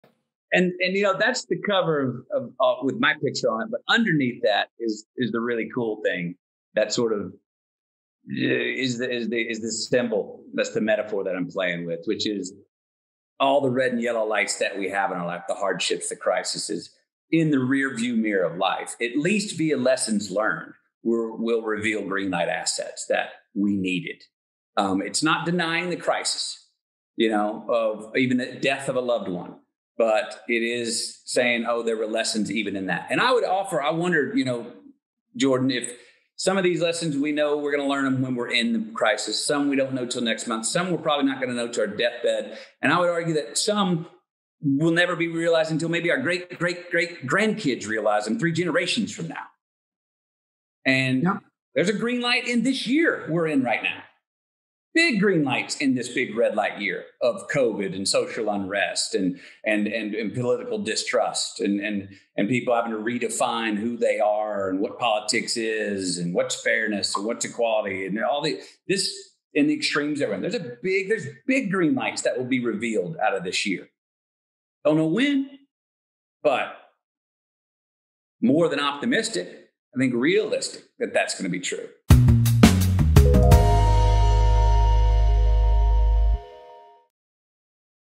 GREENLIGHTS in our life
JORDAN PETERSON and Matthew McConaughey talks about Matthew's new book Greenlight and how to understand the metaphor of greenlights.

Credit: @JordanBPeterson

This content is for educational and informational purpose.